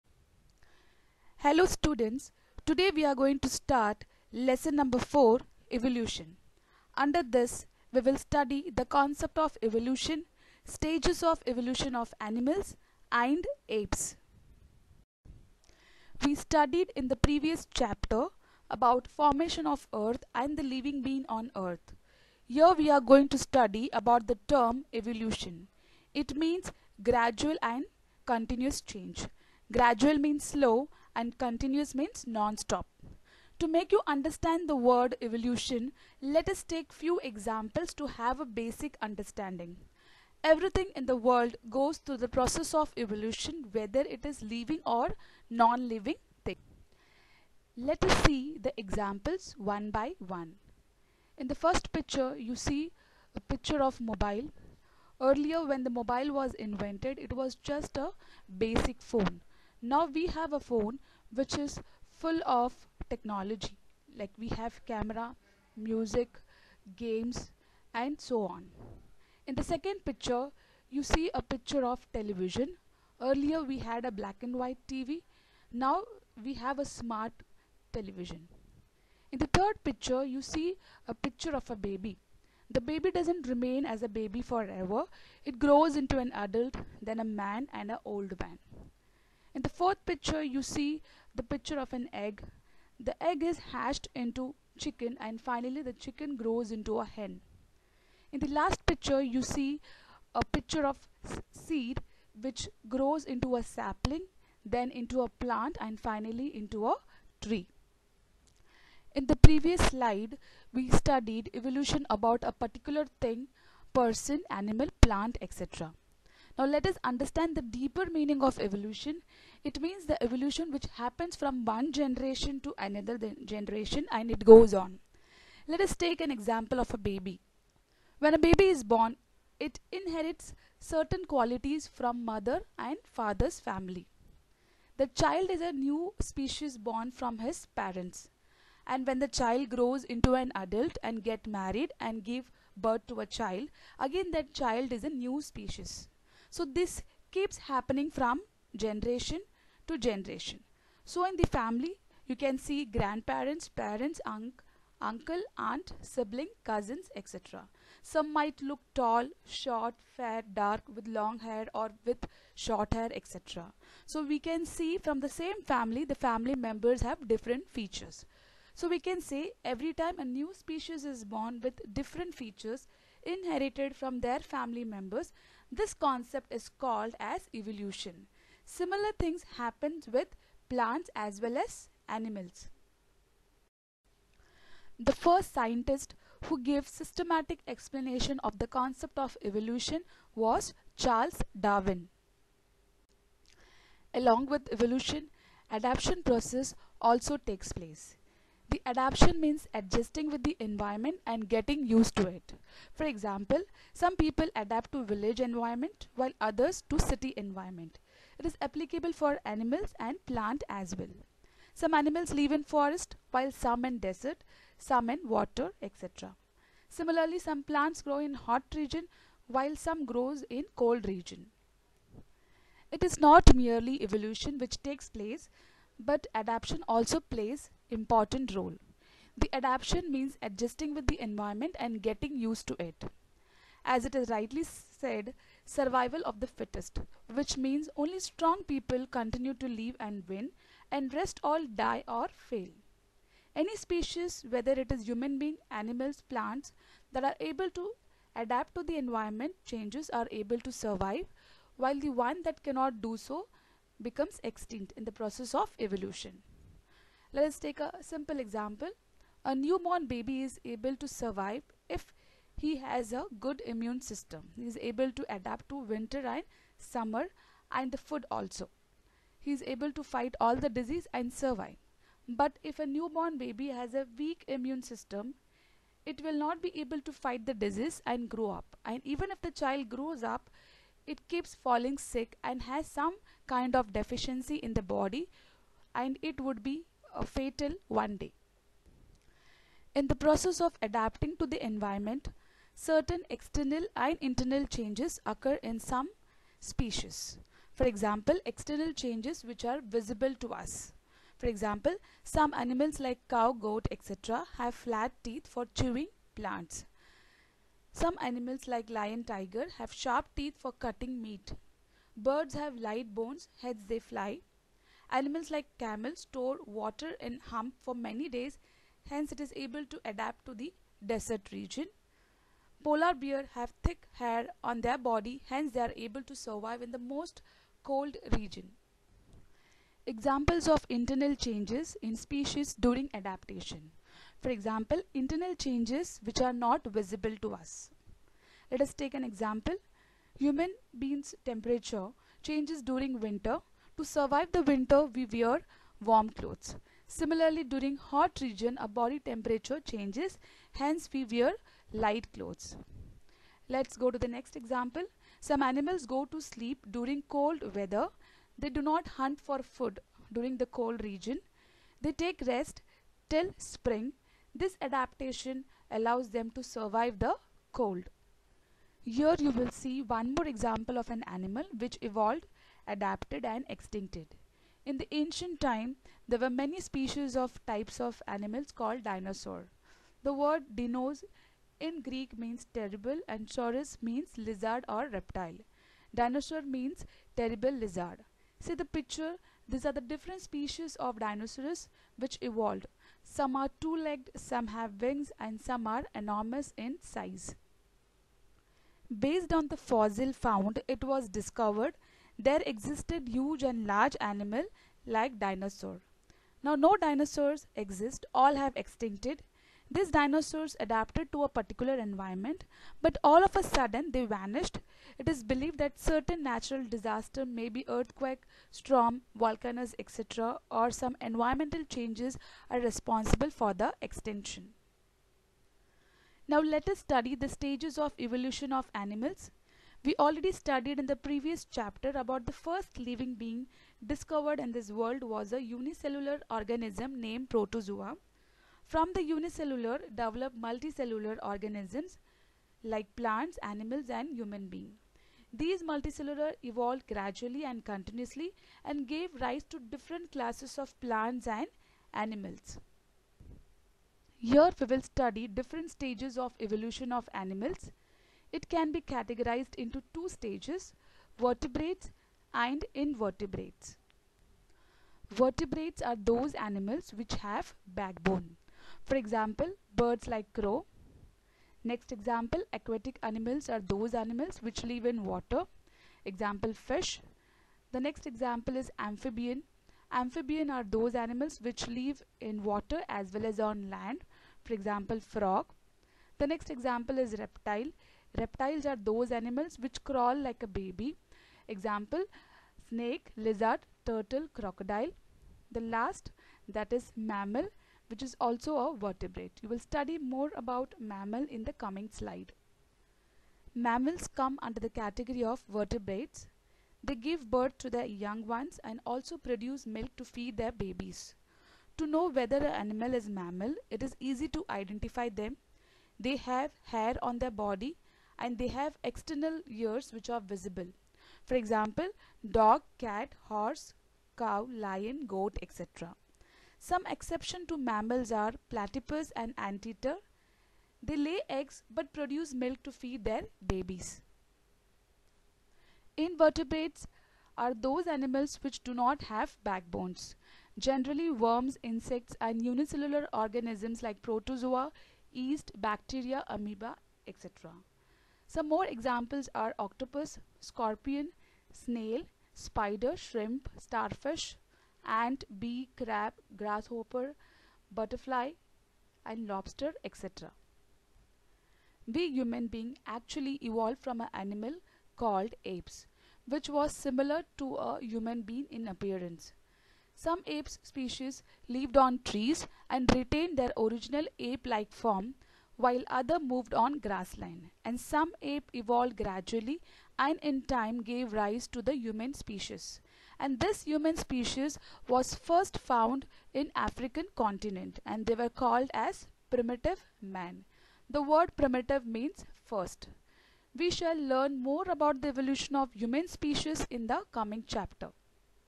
STATEBOARD_STD5_EVS2_LESSON4
In this lesson, we will studied about evolution, different stages of evolution of animals, and apes